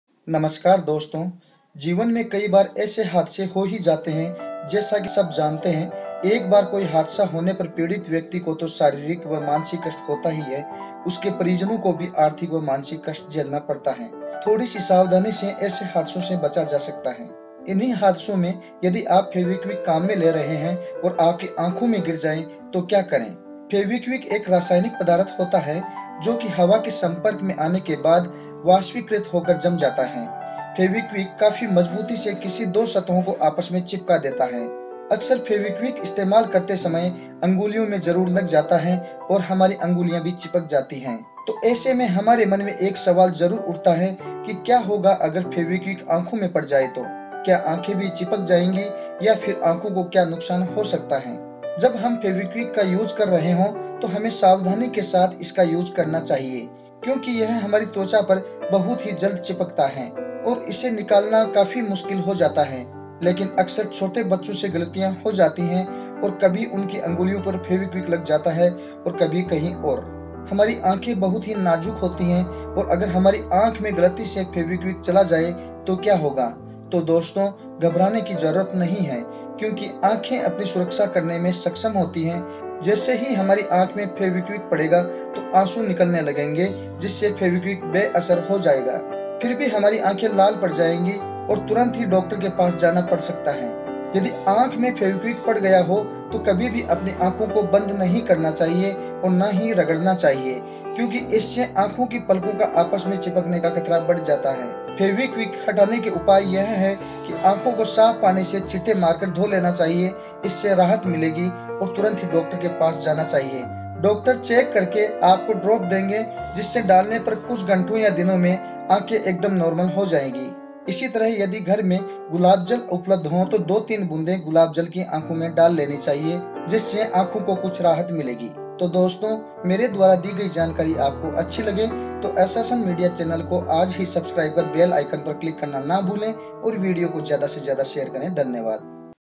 What happen if Fevikwik goes inside the eyes? Fevikwik aankh me chali jaye to kya hoga?
Fevi kwik आँखों में चली जाये तो ? What will happen  if Fevikwik goes inside the eyes?